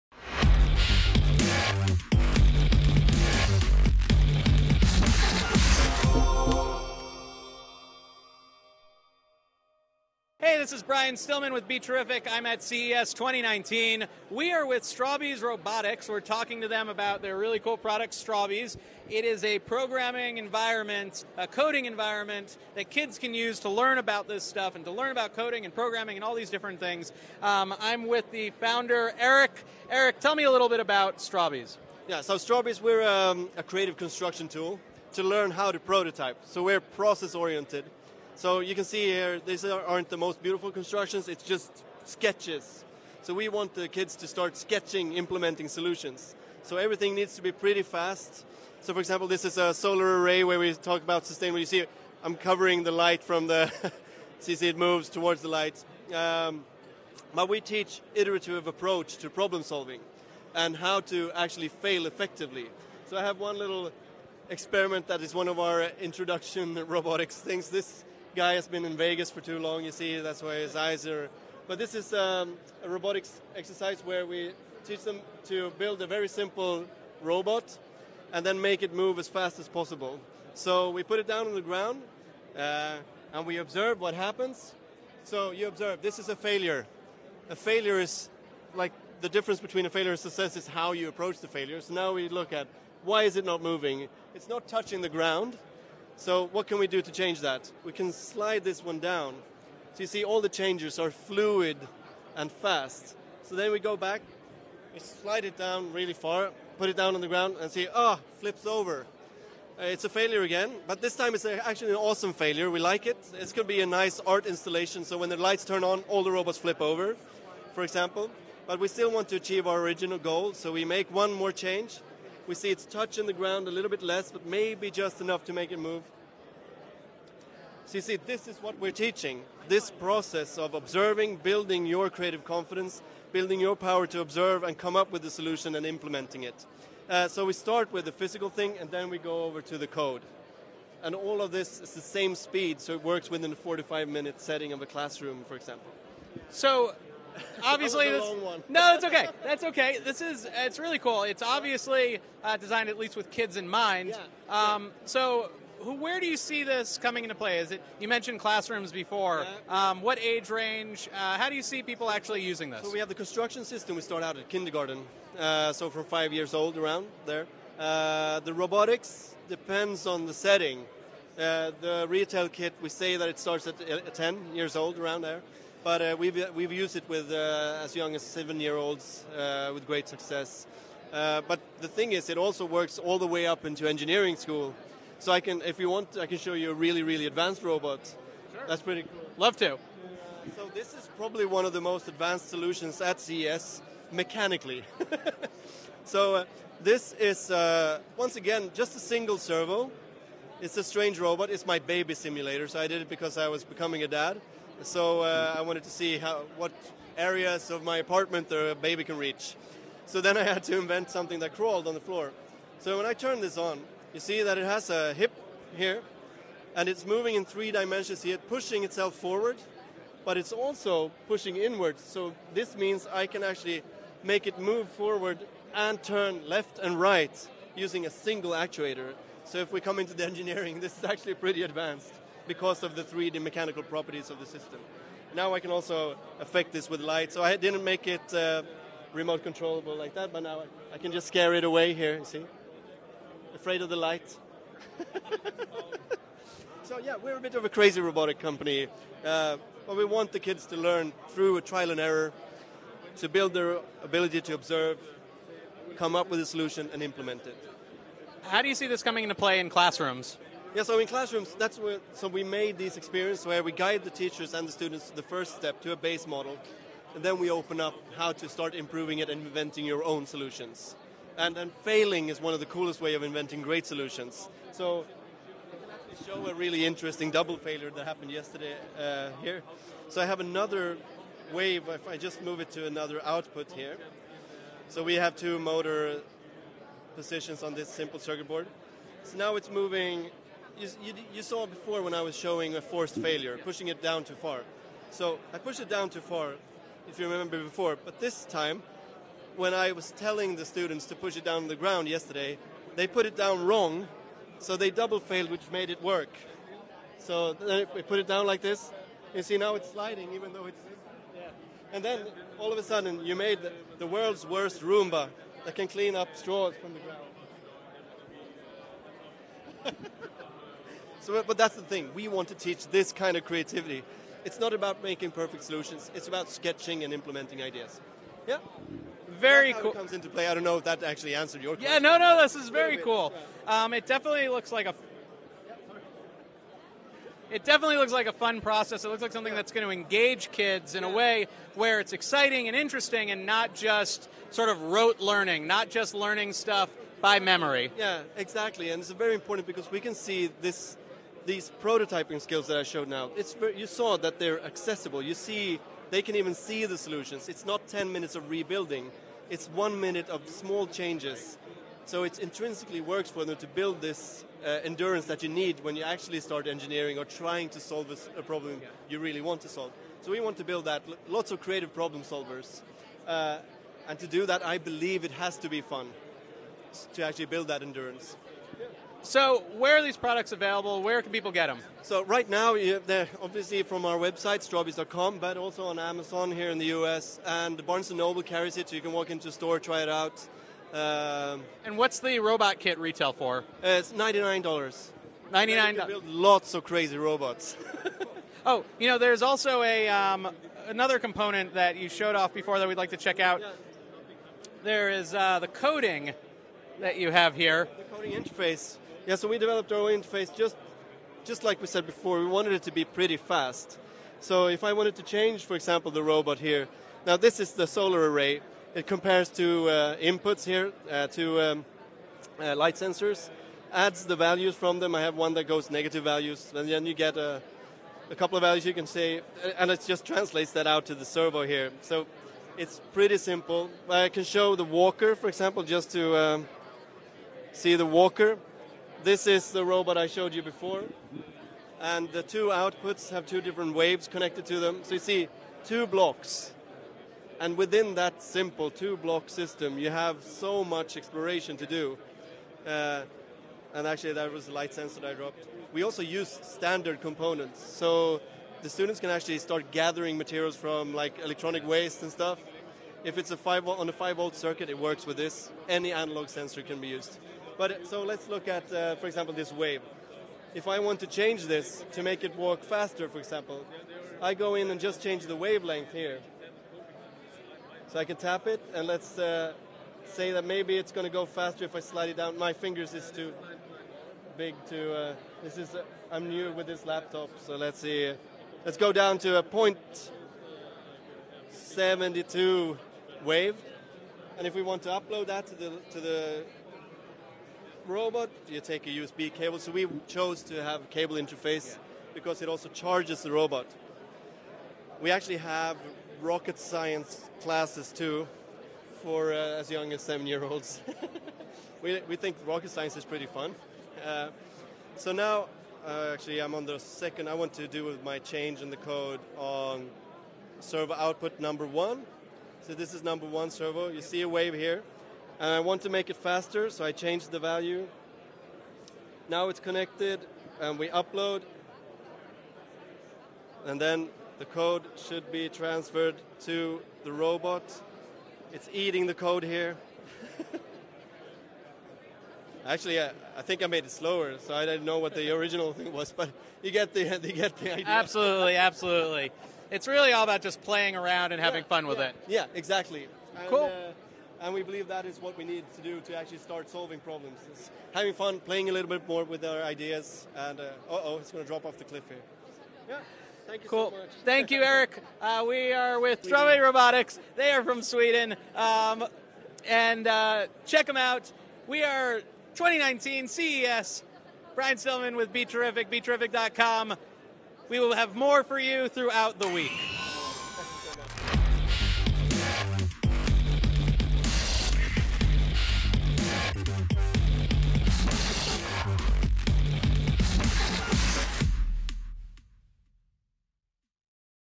The Future of Learning! STRAWBEES at CES 2019!
BeTerrific at CES 2019!

Strawbees founder Eric Torstensson takes us through the latest Strawbees prototyping kits that teaches makers of all ages coding and robotics!